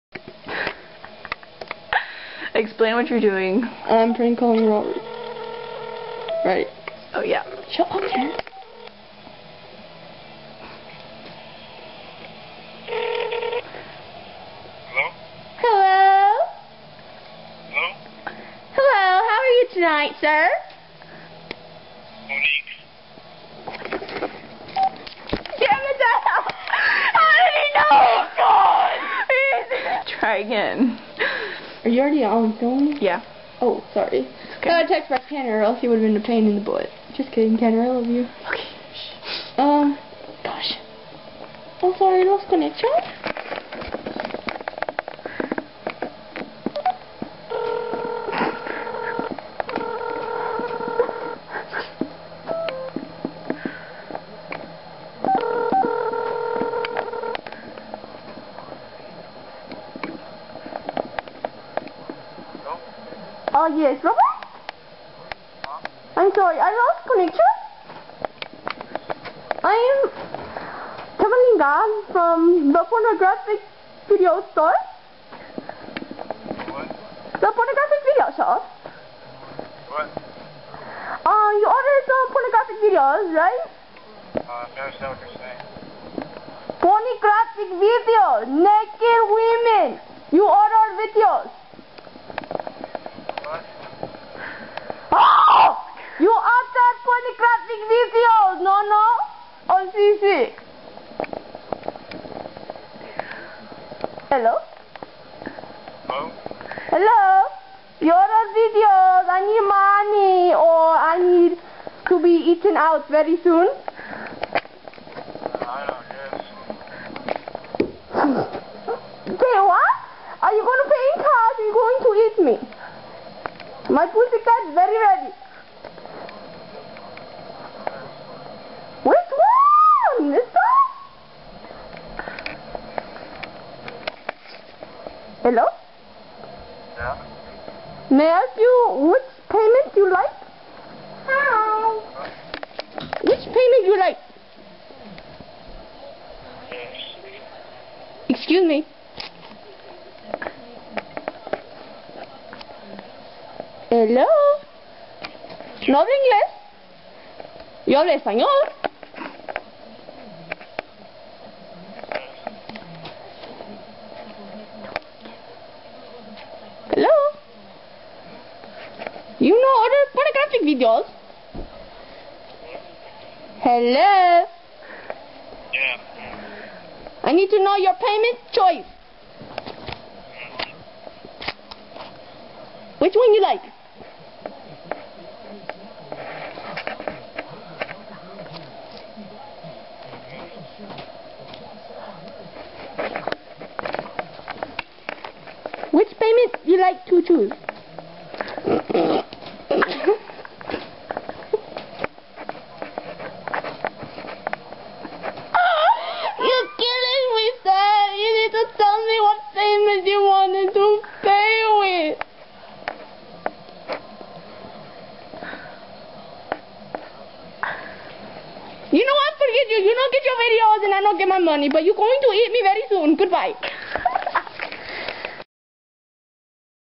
The Prank Call "The Joker" (Asian Woman Sells Porn Videos)
My bestie/wifeyy pulled a prank call on my ex boyfriend. He didn't seem to understand her.
XD